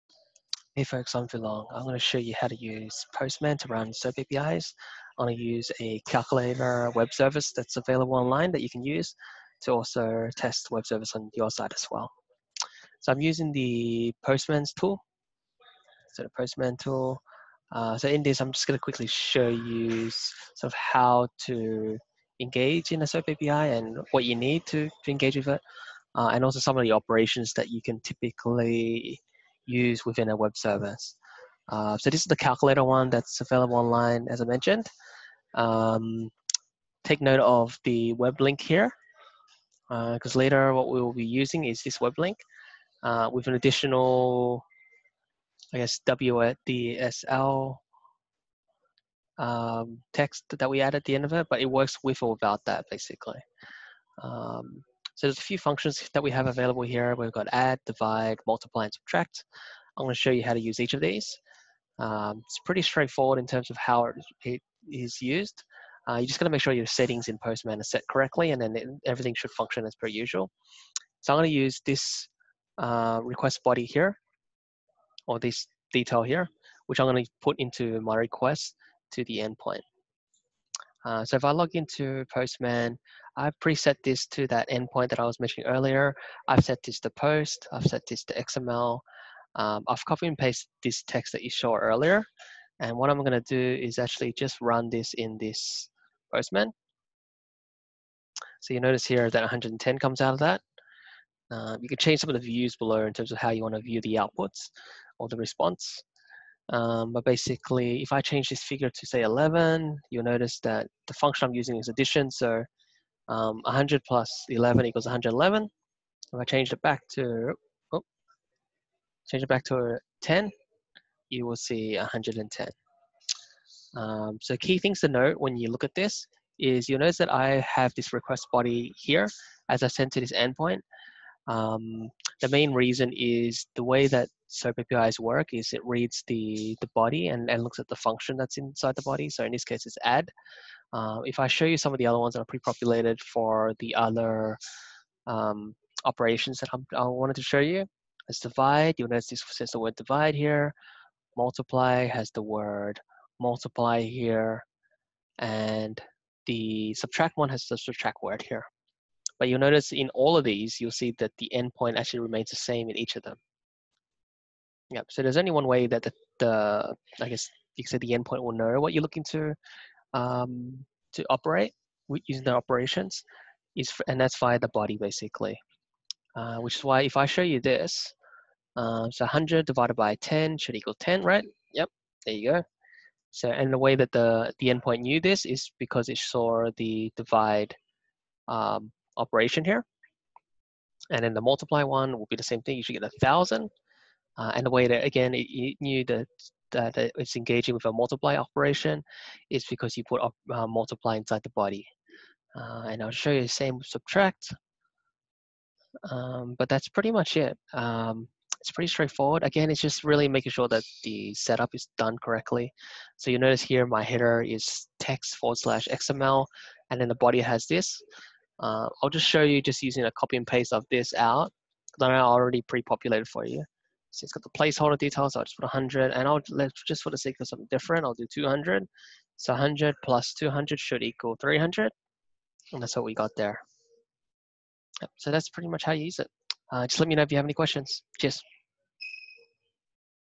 Web Services SOAP APIs - How to use Postman to test it (Calculator example)
In this video I will be showing you how to use Postman software to test your SOAP APIs and WSDL endpoints.

I will be using the online Web Services endpoint for the demonstration, Calculator.

I have a brief how to guide post here if you prefer to follow this step-by-step

You can download the free Postman software I'm using